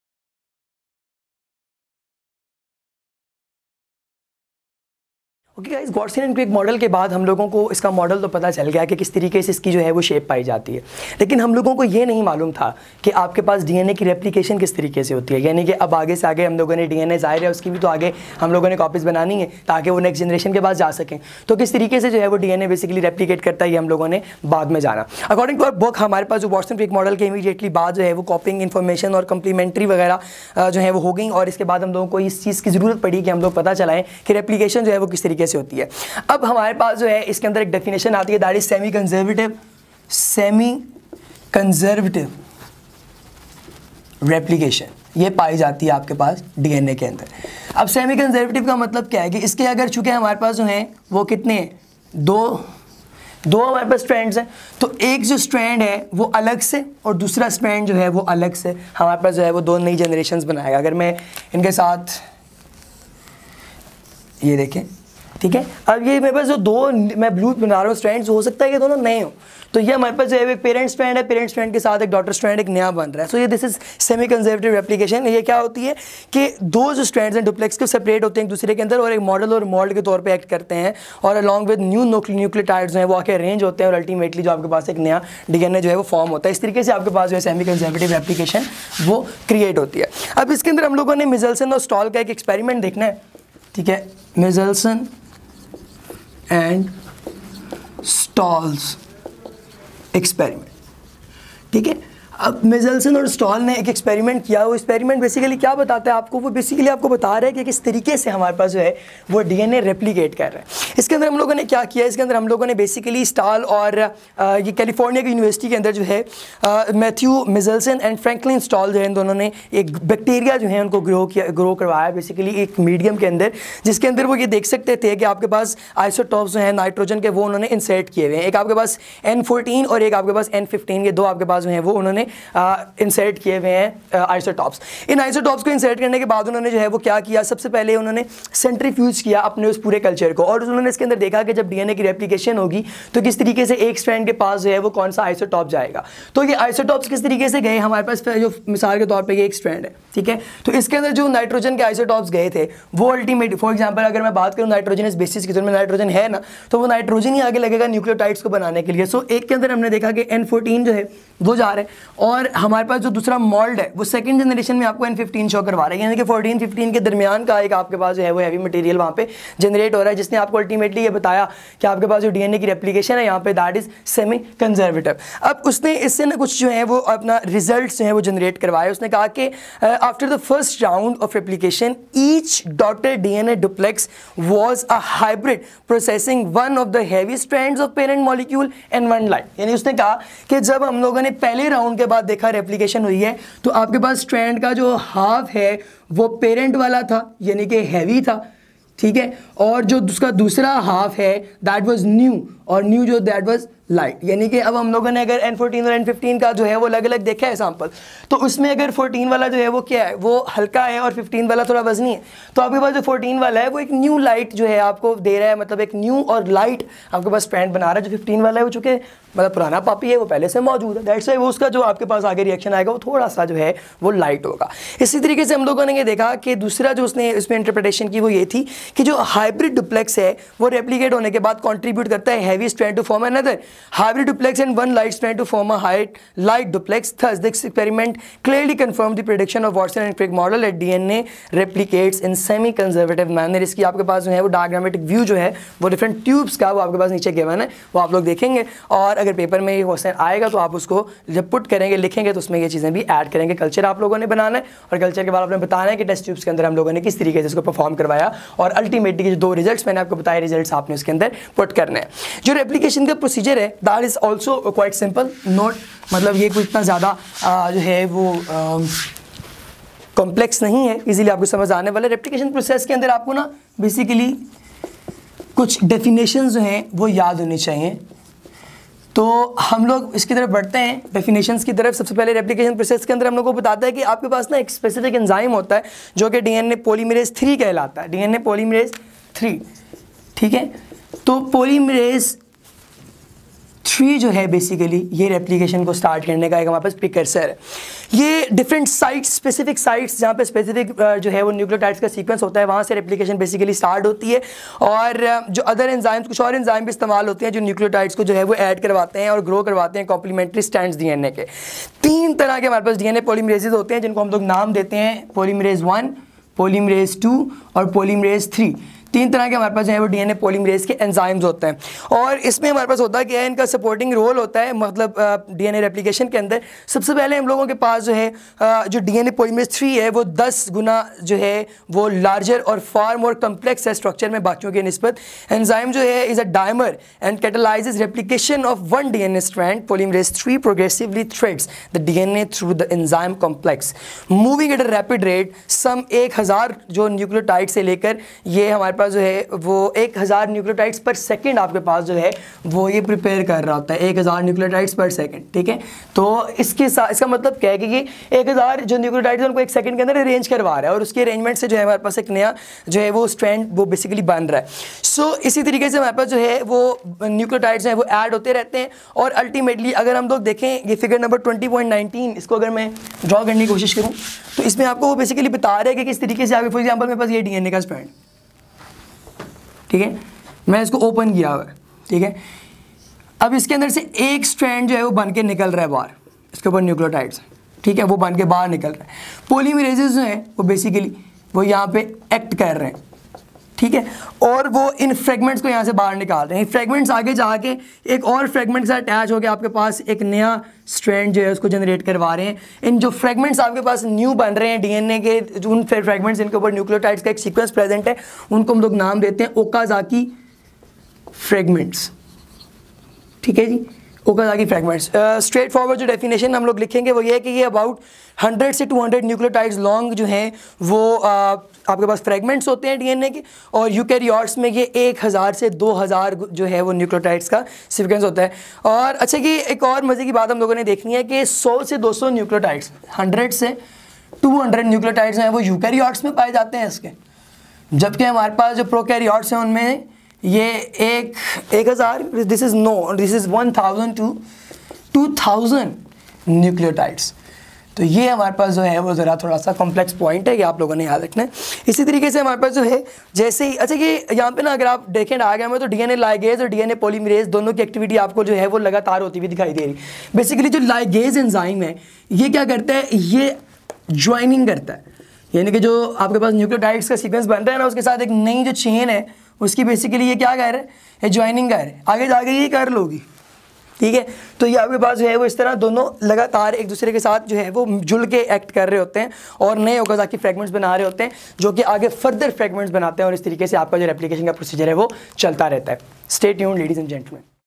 DNA Replication Biology - Biology Chapter 20 Chromosomes and DNA - FSC Part 2 Pre Medical 12th Class
ilmkidunya.com has brought to you Lecture of M.Shadab Abbasi on "12th Class Biology Chapter 20 Chromosomes and DNA.

Topic 5 DNA Replication".

In this video following sub topics have been taught:

- Meselson-Stahl’s Experiment
- Replication Process

This lecture is specially recorded for students of Intermediate Part 2, FSc Pre Medical Part 2 from all Punjab Boards and is based on the current curriculum of study for Biology book.